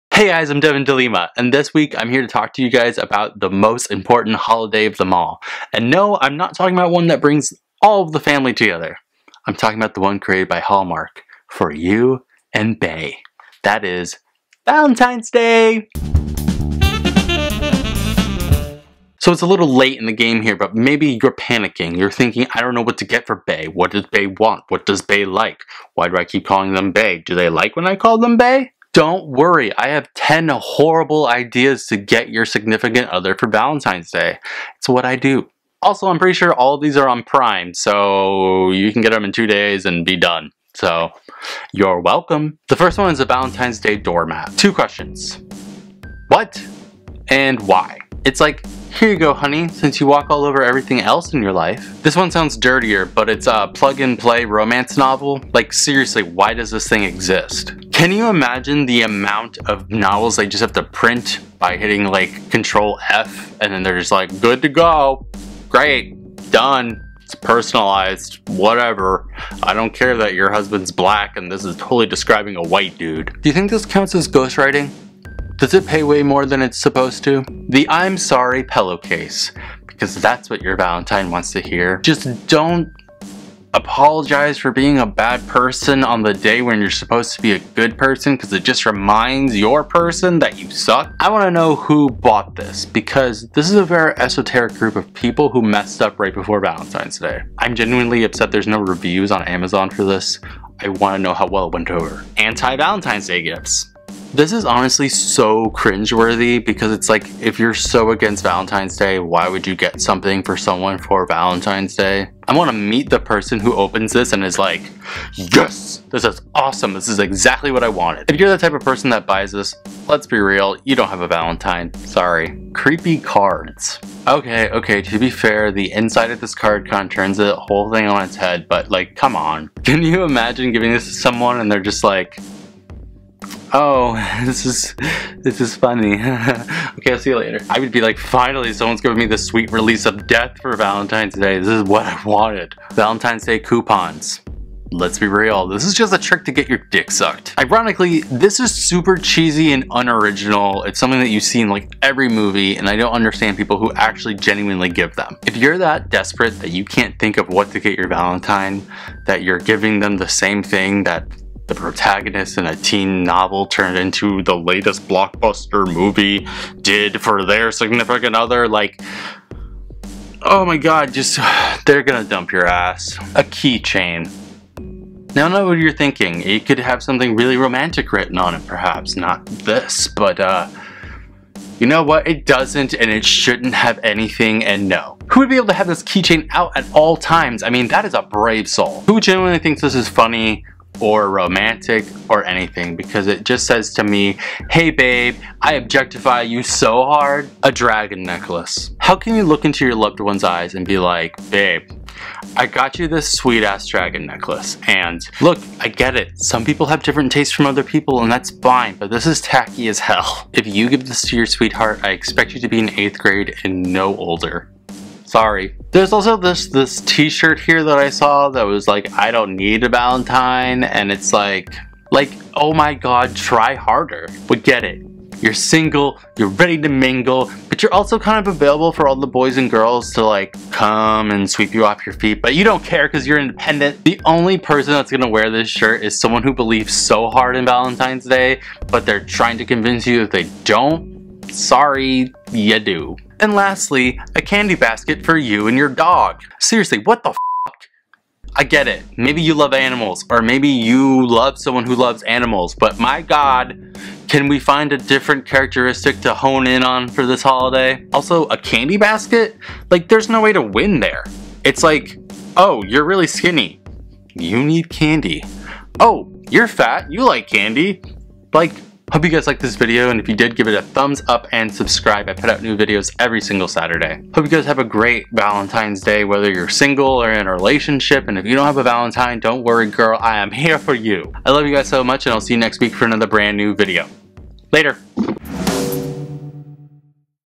Worst Valentines Day Gifts EVER 2017 || Devin de Lima
Valentines right around the corner, and no ideas for bae? It's 2017! Get to online shopping for Valentines Day!

Equipment -
Camera: Canon Vixia HF R52
Microphone: Røde VideoMic
Lights: Cowboy Studio Softbox Lightning x 2 (600 Watt)
Editing: Final Cut Pro, Adobe After Effects